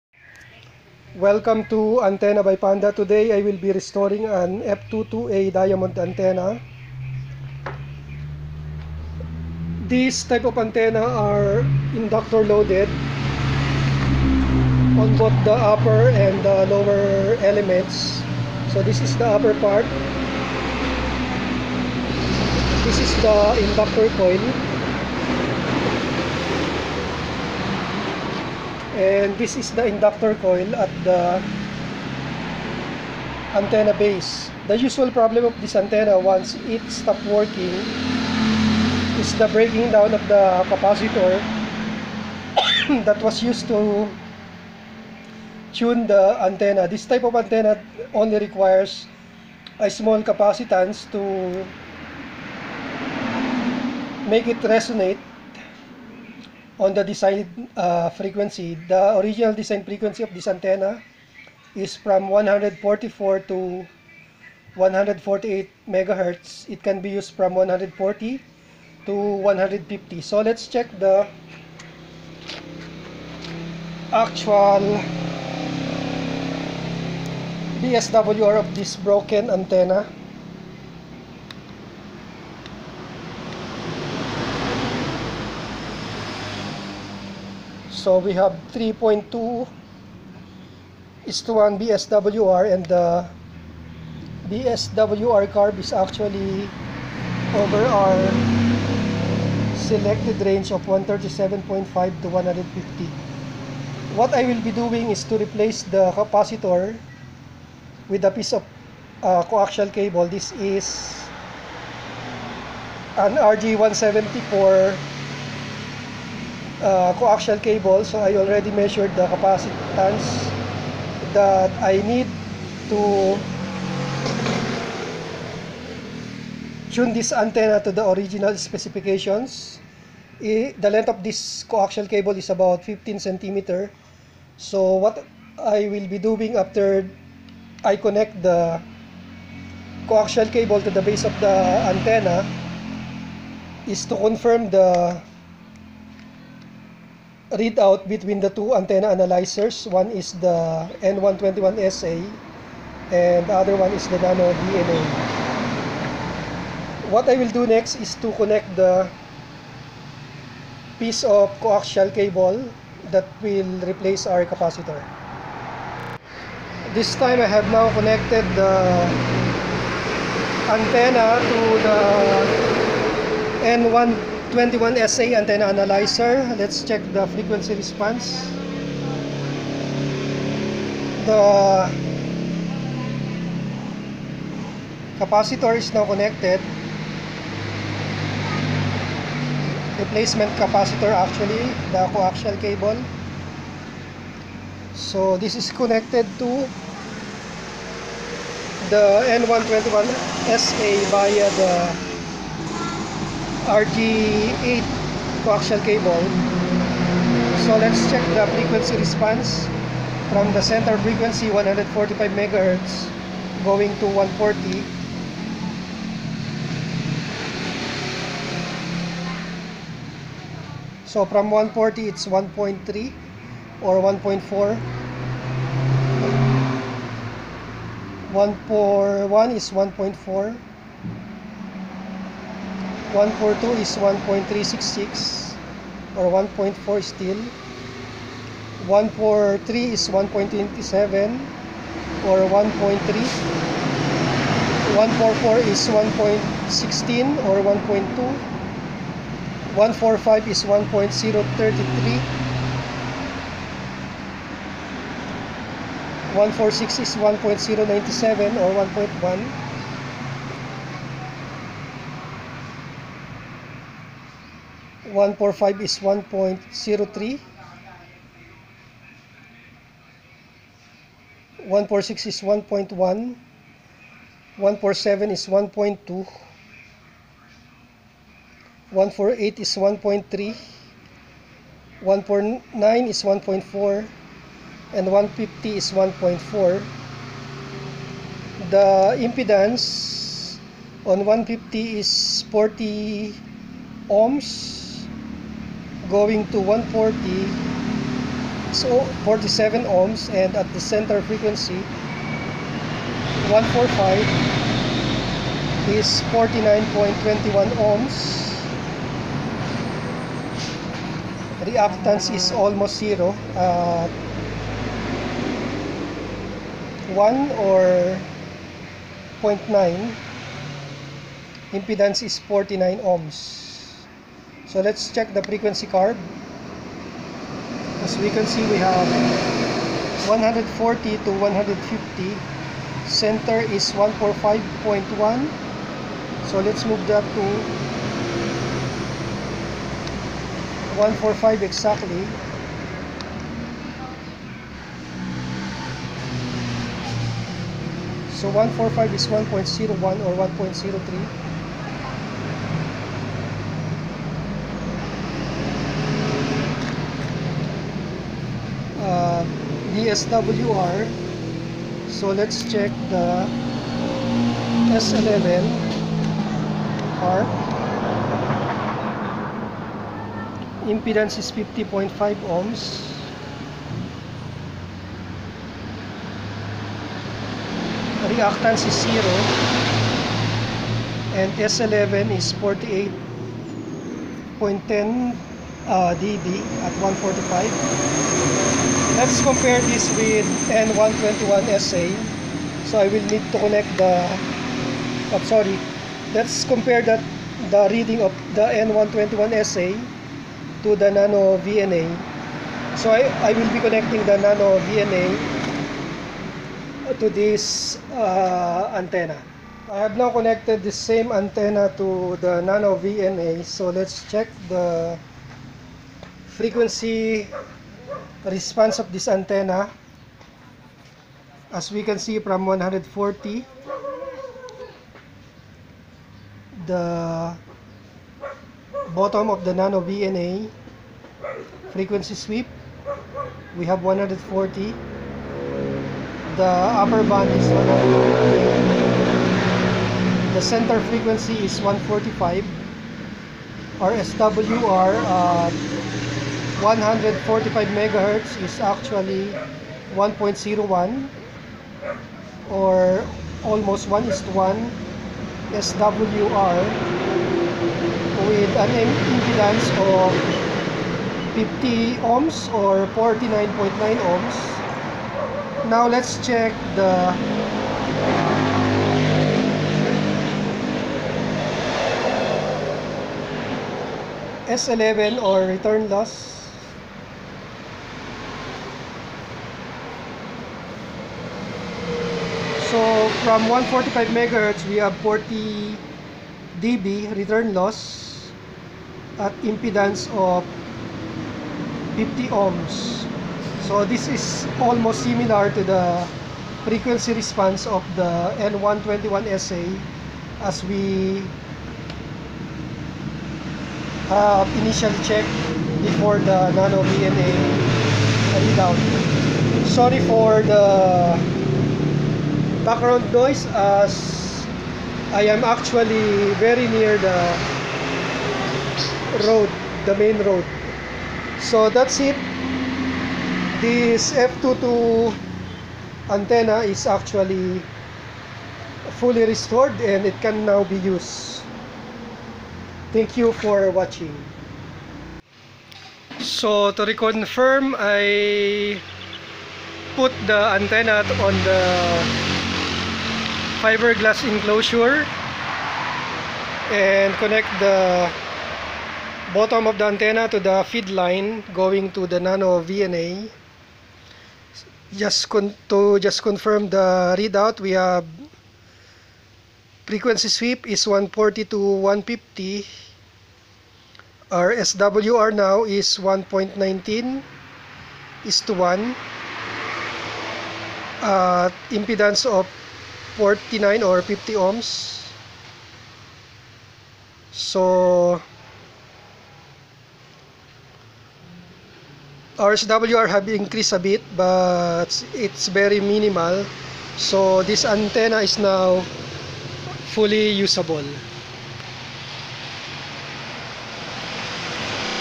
Restoring Diamond F22A antenna coax replacement for capacitor - calibrated with N1201SA and NanoVNA
Diamond F22A antenna restoration, by replacing the loading capacitor with a portion of RG174 coaxial cable. Antenna frequency calibration was done on N1201SA and NanoVNA antenna analyzers.